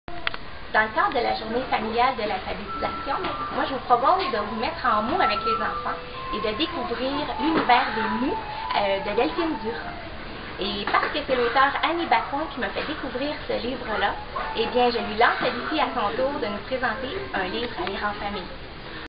Défi de l'alphabétisation familiale : May présente
La journée de l'alphabétisation familiale au Canada (le 27 janvier 2017)

May vous propose Les fabuleuses aventures des mous de Delphine Durand.

May lance le défi de présenter un livre à Annie Bacon.

Vous pouvez nous faire part de vos commentaires et de vos suggestions de thématiques.